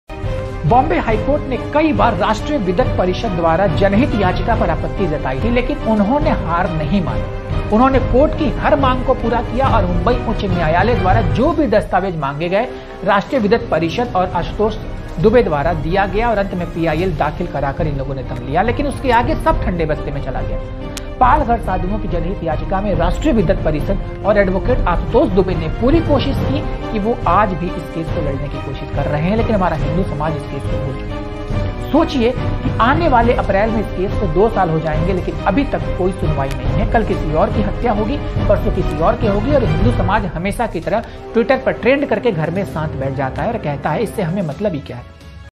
Palghar Sadhu Lynching Case / Advocate Ashutosh Dubey / Rashtriya Vidwat Parishad / CBI Enquiry
याचिकर्ता के अधिवक्ता आशुतोष दुबे से मिली जानकारी के मुताबिक पालघर साधु मामले में 28/09/2020 को राष्ट्रीय विद्वत परिषद द्वारा याचिका की सुनवाई हुई थी जिसको आज पूरे दो साल हो गए और माननीय उच्च न्यायालय ने पीआईएल को अन्य 6 पीआईएल के साथ कोर्ट के समक्ष स्वीकार कर वकील – आशुतोष दुबे याचिका को भी जोड़ दिया था लेकिन महाराष्ट्र सरकार द्वारा सर्वोच्चन्यायालय में याचिकाओं पर रोक लगाने की वजह से उचच न्यायालय ने आदेश दिया कि सर्वोच्चन्यायालय के अगले आदेश तक इस याचिका को अगले तारीख तक टाला जाता है।

उन्होंने आगे कहा कि बॉम्बे हाई कोर्ट में “RASHTRIY VIDWAT PARISHAD, दिल्ली” की ओर से वकील – आशुतोष दुबे ने PIL दायर किया था । याचिका दाखिल कर पालघर घटना की कोर्ट की निगरानी में सीबीआई जांच की मांग की गई थी और साथ ही मामले को महाराष्ट्र से दिल्ली ट्रांसफर कर दिल्ली में ट्रायल चलाने की गुहार लगाई गई थी ।